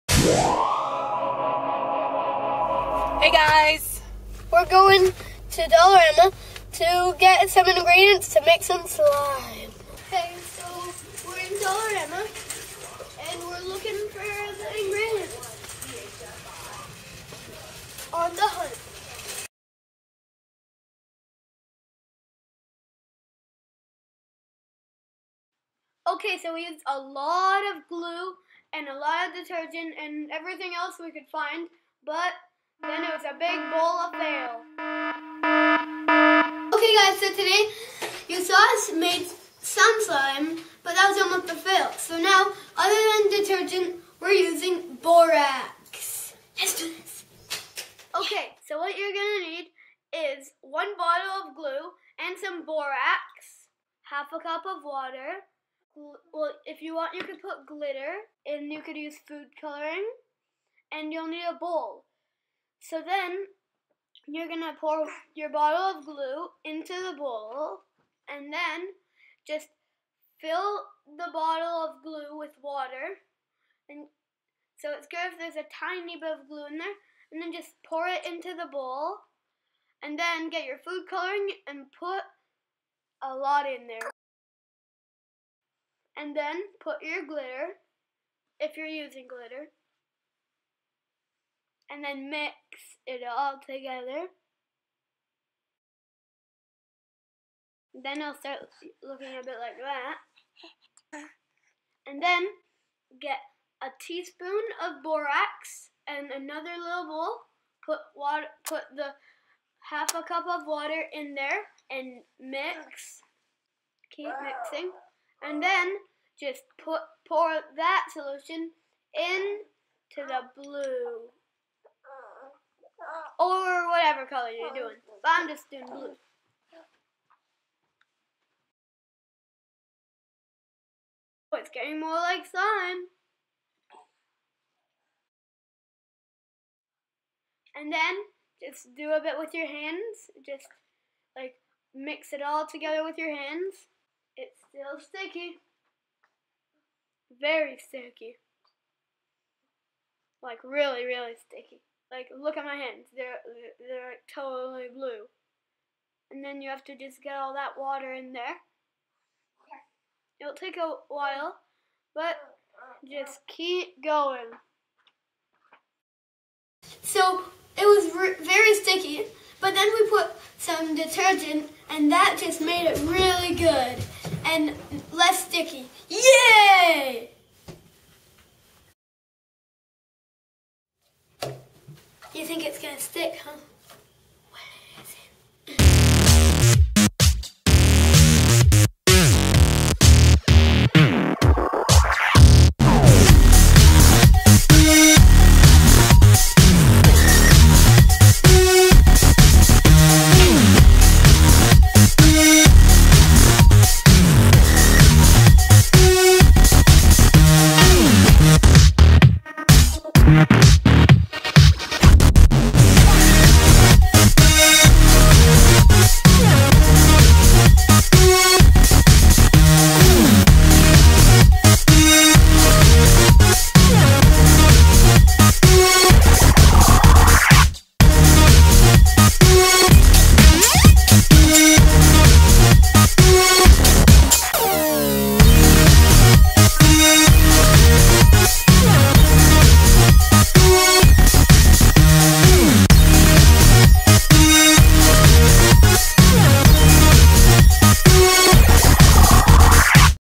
MAKING SLIME with Freaky Camel Zak D.I.Y.
Learn to make awesome SLIME with Zak!

what you need:
- 1 bottle of white glue
- 1/2 cup warm water
- 1 tsp. borax (find it in the laundry aisle)
- little splash of laundry detergent
- food colouring and/or sparkle (if desired)

Watch for more slime experiments with Zak coming soon!